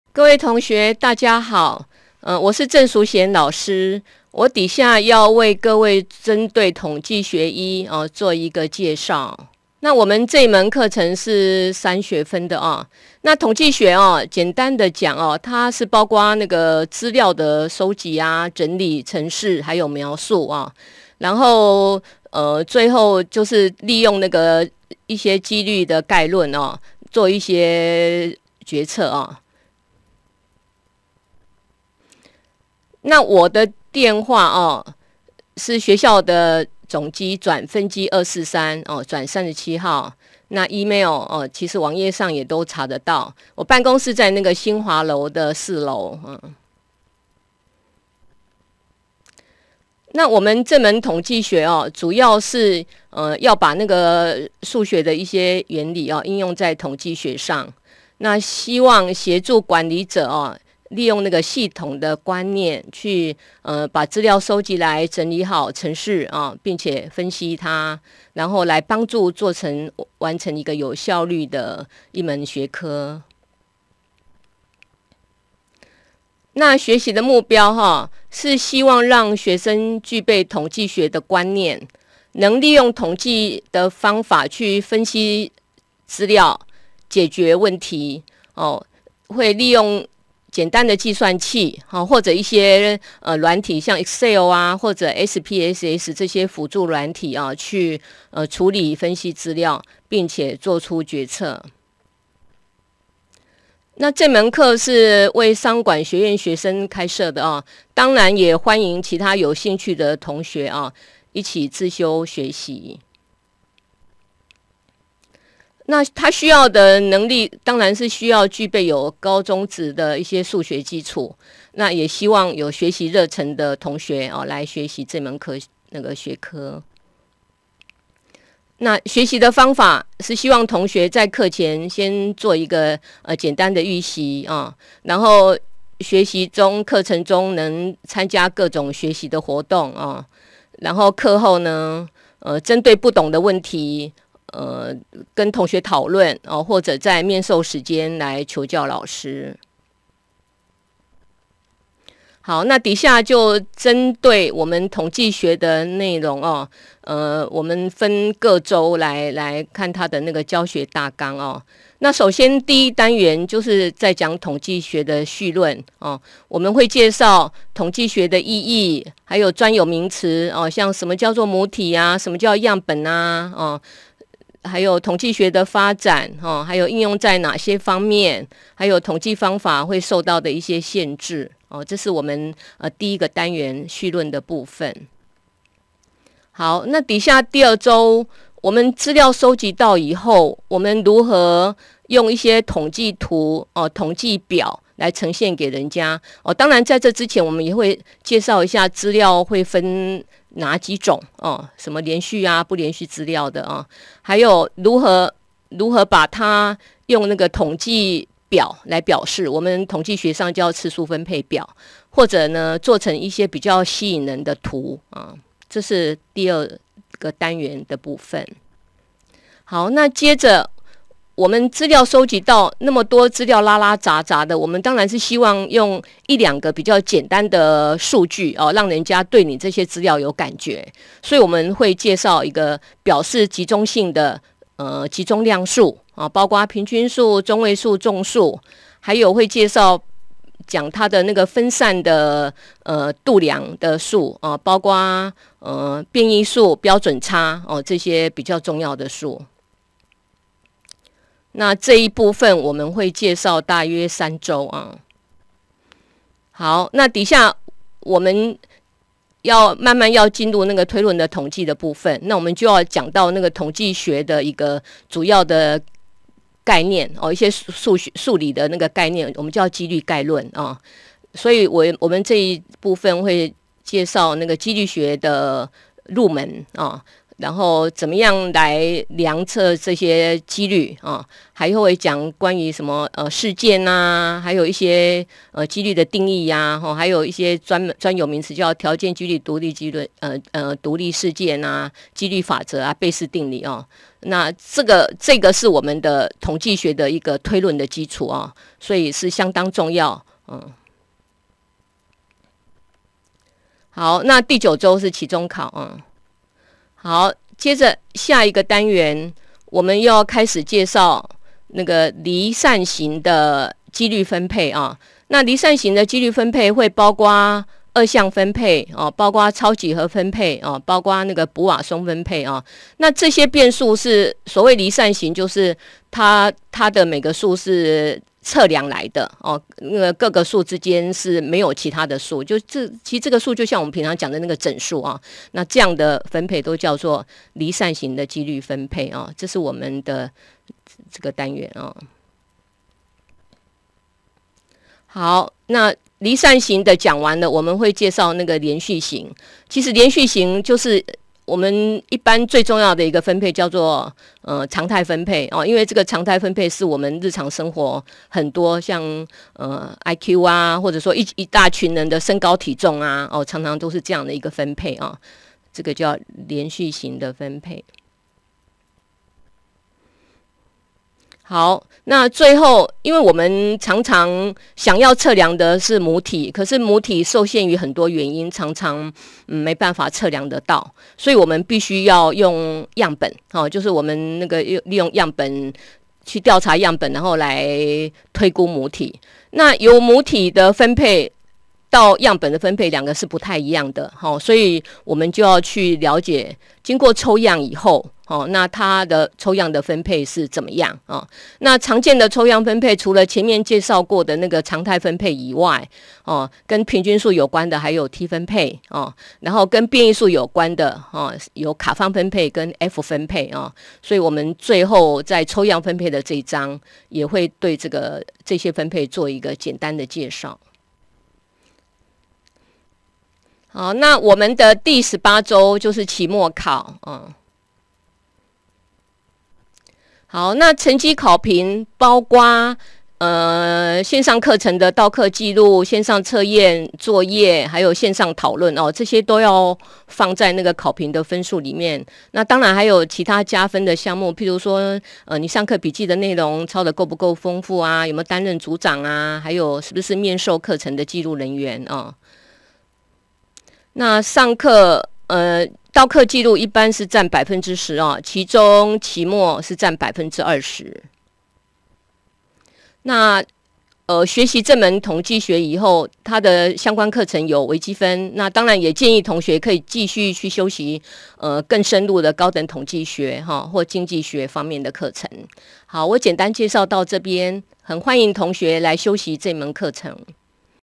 統計學一-教師及課程簡介-鄭淑賢
課程簡介：統計學一課程由鄭淑賢老師講授，本單元為各位介紹授課教師與課程進行方式。
課程難度：普通
適合對象：一般大眾
授課教師：鄭淑賢
製作單位：中華科技大學 遠距教學組
製作管理：林文博
製作助理：郭恬君